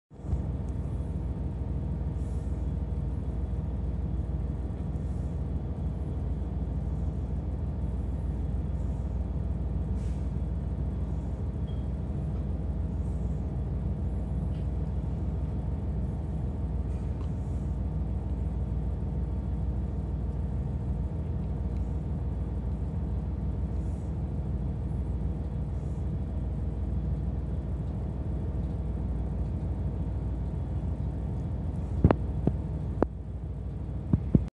キハ189系 車内表示
2018年7月～9月までの土曜・休日に米子―鳥取間で運行していた 臨時特急大山号に乗車した時です